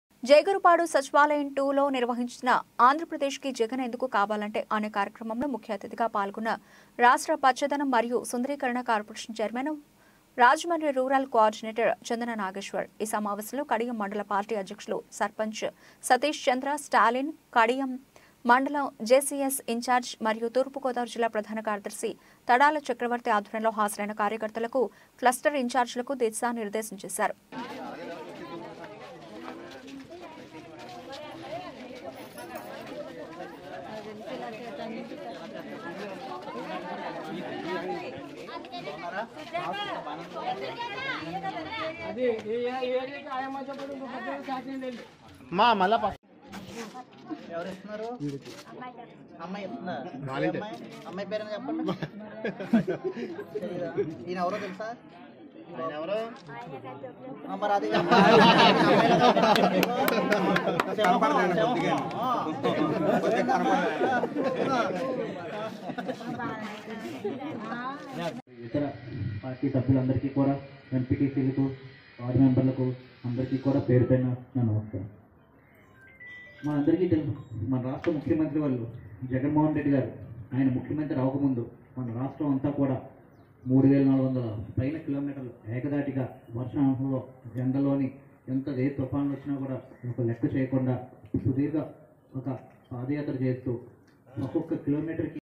ABC NEWS TELUGU//ఏపీకి జగనే ఎందుకు కావాలి..ఏపీ నీడ్స్‌ జగన్‌ కార్యక్రమంలో రూరల్ కోఆర్డినేటర్ చందన.
- ఏపీకి జగనే ఎందుకు కావాలి..

-సుపరిపరిపాలన కోసం జగనన్న కావాలి..

-ఏపీ నీడ్స్‌ జగన్‌ కార్యక్రమంలో దిశా నిర్దేశం

జేగురుపాడు.. నవంబరు..15-11-23.

 జేగురుపాడు సచివాలయం-2. లో నిర్వహించిన 'ఆంధ్రప్రదేశ్ కి జగన్ ఎందుకు కావాలంటే.'అనే కార్యక్రమంలో ముఖ్య అతిథిగా పాల్గొన్నా  రాష్ట్ర పచ్చదనం మరియు సుందరీకరణ కార్పొరేషన్ చైర్మన్ మరియు రాజమండ్రి రూరల్ కో ఆర్డినేటర్  చందన నాగేశ్వర్.  ఈ సమావేశం లో కడియం మండల పార్టీ అధ్యక్షులు,సర్పంచ్ సతీష్ చంద్ర స్టాలిన్,కడియం మండలం జెసియస్ ఇన్చార్జి మరియు తూర్పుగోదావరి జిల్లా ప్రధాన కార్యదర్శి తడాల చక్రవర్తి ఆధ్వర్యంలో హాజరైన కార్యకర్తలకు, క్లస్టర్‌ ఇంచార్జ్‌లకు దిశానిర్ధేశం చేశారు.

జనం కోసం, రాష్ట్రం కోసం అనునిత్యం పరితపించే నిజమైన నేత ముఖ్యమంత్రి జగన్మోహన్ రెడ్డి అని, అందుకే మరోసారి మరోసారి.. ఈ రాష్ట్రానికి సీఎంగా జగన్మోహన్ రెడ్డే ఉండాలని అన్ని వర్గాల ప్రజలు ముక్త కంఠంతో చెబుతున్నారని రాష్ట్ర పచ్చదనం మరియు సుందరీకరణ కార్పొరేషన్ చైర్మన్ మరియు రాజమండ్రి రూరల్ కో ఆర్డినేటర్  చందన నాగేశ్వర్  తెలిపారు. బుధవారం జేగురుపాడు సచివాలయం-2 'ఆంధ్రప్రదేశ్ కి జగనే ఎందుకు కావాలంటే'..అన్న అంశంపై అవగాహనా సమావేశం నిర్వహించే ముందుగా పార్టీ జెండాను ఆవిష్కరించారు.

ఈ సమావేశంలో చందన నాగేశ్వర్ మాట్లాడుతూ... నిరంతరం పేదల కోసం పరితపించే మానవతామూర్తి జగనన్న అని, అటువంటి నాయకుడు పది కాలాలపాటు సీఎంగా ఉంటే ఆంధ్రప్రదేశ్ రాష్ట్రం దేశంలో నెంబర్ వన్ స్టేట్ గా అభివృద్ధి చెందుతుందన్నారు. స్వాతంత్ర్యం వచ్చిన ఏడున్నర దశాబ్దాల కాలంలో ఎందరో పాలకులు వచ్చారు, వెళ్ళారు.ఏ ఒక్కరూ రాష్ట్రాభివృద్ధికి గానీ, ప్రజా సంక్షేమాన్ని గానీ పట్టించుకోలేదని అన్నారు.అన్నివర్గాల ప్రజలకి సంక్షేమ పథకాలు అందాలంటే రాష్టానికి  జగన్నన్న కావాలన్నారు. గతంలో ఎప్పుడు లేనంతంగా సంక్షేమ పధకాల వెల్లువ రాష్ట్రంలో జరిగిందన్నారు. ప్రతి ఒక్కరికీ సంక్షేమ పధకాలు అందాయాని లబ్దిదారులే ఆనందాన్ని వ్యక్తం చేస్తున్నారన్నారు. సీఎం జగనన్న అందించిన పధకాలు గురించి ఇంటింటికి వెళ్లి అందరికీ మరోసారి గుర్తు చేయాలన్నారు. మళ్లి సీఎంగా జగనన్న వస్తే మరన్ని సంక్షేమ పధకాలు పేదలకు, అర్హులకు అందుతాయాని తెలియచేయలన్నారు. మేలు జరిగితేనే ఓటు వేయ్యండి అని సీఎం జగన్‌ చేబుతున్నారని దీనిని బట్టి రాష్ట్రంలో ప్రజలకు ఎంత మేలు జరిగిందో తెలుసుకోవచ్చన్నారు.ఈ విషయాలన్ని క్లస్టర్‌ అధ్యక్షులు, ఇంచార్జ్‌లు ప్రజలలోకి తీసుకుని వెళ్లాలన్నారు. రాష్ట్రానికి స్వర్ణయుగం కావాలంటే మళ్లి సీఎంగా జగనన్నే రావాలన్నారు. వైఎస్సార్ సీపీ అధికారంలోకి వచ్చాకఈనాలుగున్నరసంవత్సరాలలో జేగురుపాడు గ్రామాన్ని 21 కోట్ల 93 లక్షలతో ఎంత అభివృద్ధి చేశామో ప్రజలు స్వయంగా చూసి అభినందిస్తున్నారని, గృహ సారధులు, కన్వీనర్లు రోజుకు 10  కుటుంబాలను కలిసి జగనన్న సంక్షేమ పథకాలని వివరించాలని అన్నారు.

ఈ సమావేశంలో రాజమండ్రి కడియం మండల పార్టీ అధ్యక్షులు, సర్పంచ్ సతీష్ చంద్ర స్టాలిన్,కడియం మండలం జెసియస్ ఇన్చార్జి మరియు తూర్పుగోదావరి జిల్లా ప్రధాన కార్యదర్శి  తడాల చక్రవర్తి,  వీరా శివాజీ, మాజీ ఎంపీటీసీ, పుట్టా సత్యనారాయణ, కన్వీనర్, వరసాల మాధవరావు కన్వీనర్,  వైఎస్సార్సీపీ  నాయకులు,గృహసారధులు, వార్డు వాలంటీర్లు, సచివాలయ సిబ్బంది,వైఎస్సార్సీపీ కార్యకర్తలు తదితరులు పాల్గొన్నారు.